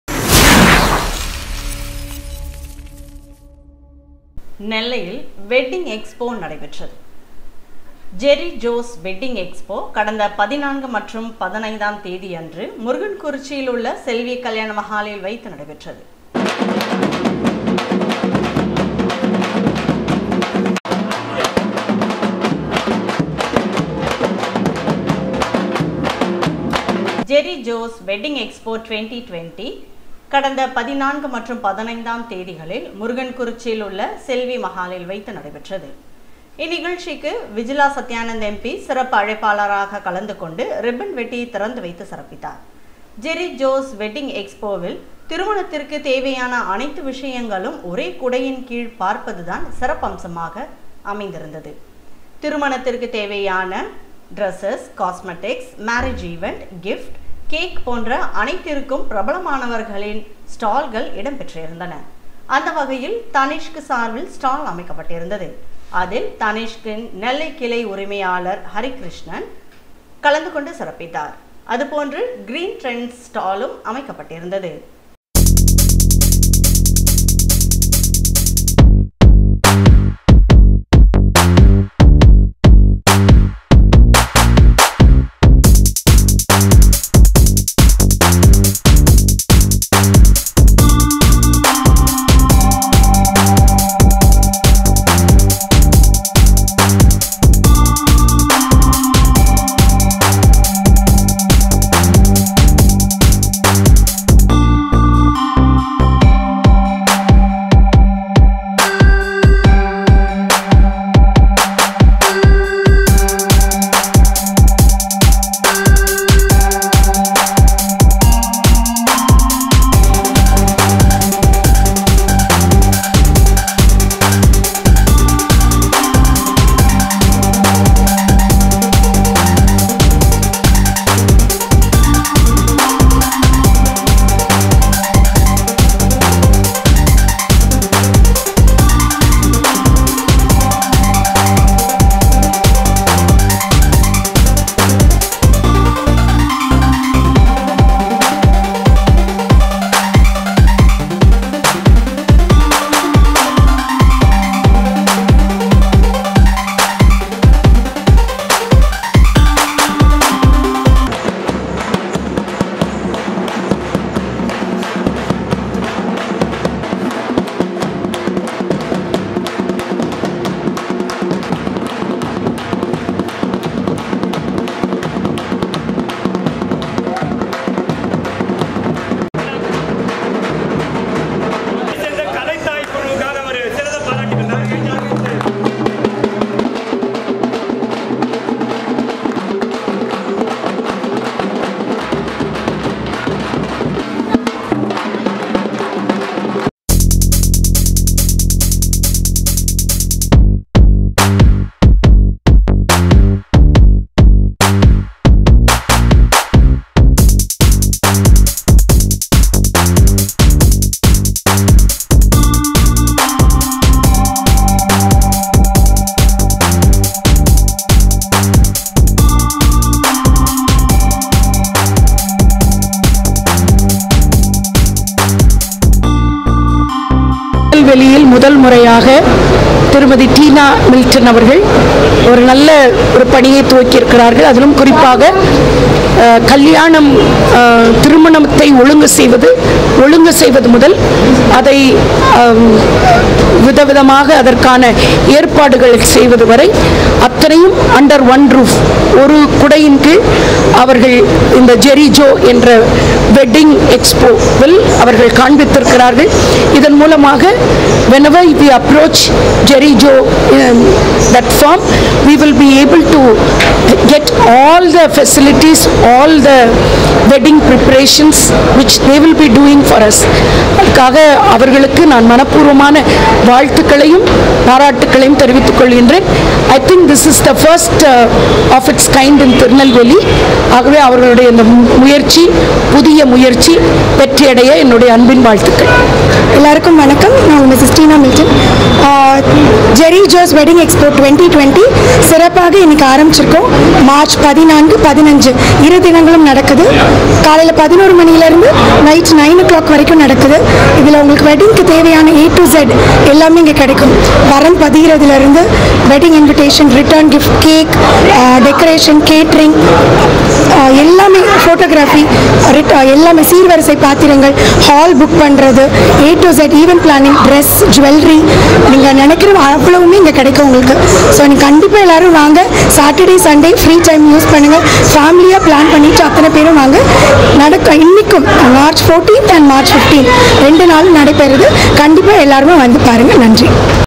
JERY JO'S WEDDING EXPO 2020
Mayuritv mayilosai TIRUNELVELI TENKASI AMBASAMUDRAM பாவூர்ச்சத்திரம் நெல்லை# JERY JO'S WEDDING EXPO 2020 செல்வி மஹாலில் 14 மற்றும் 15 ஆகிய இறுதினங்களில் நடைபெற்றது. இதில் சிறப்பு அழைப்பாளராக விஜிலா சத்யானந்த் MP கலந்து கொண்டு ரிப்பன் வெட்டி திறந்து வைத்தார். திருமணத்திற்கு தேவையான அனைத்தும் ஒரே குடையில் கீழ் பார்த்தது தான் இந்த சிறப்பம்சமாக கருதப்படுகிறது.